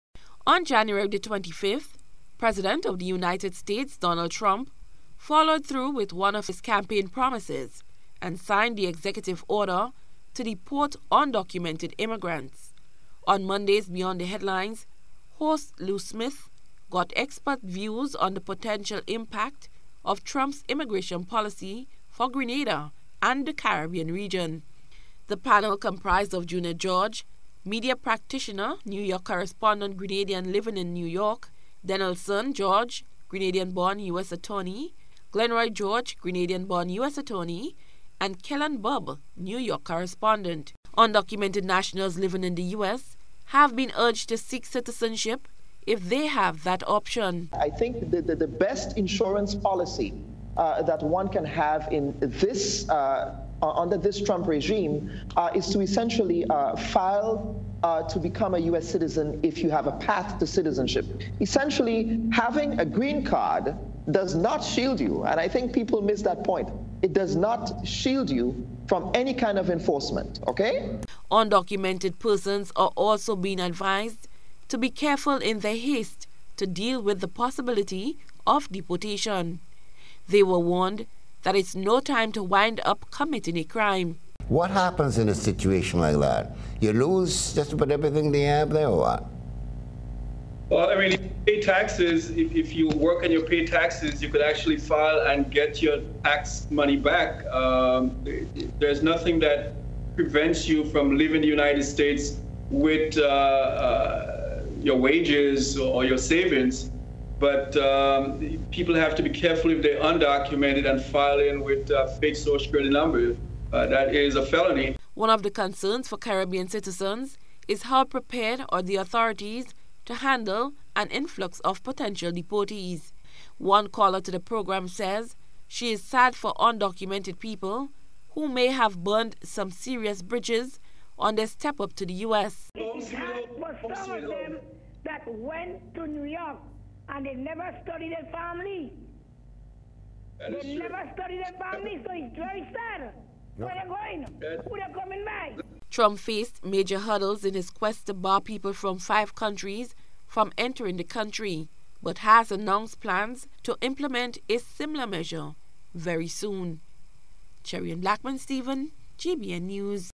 Grenadians Urged To Abide By The Law
Grenadians in the United States are being told to play it safe now more than ever.
Lawyers and others warn them that now is not the time to even run a traffic light.
They are being warned that committing the most mundane illicit act puts them at risk of being forced out of the U.S.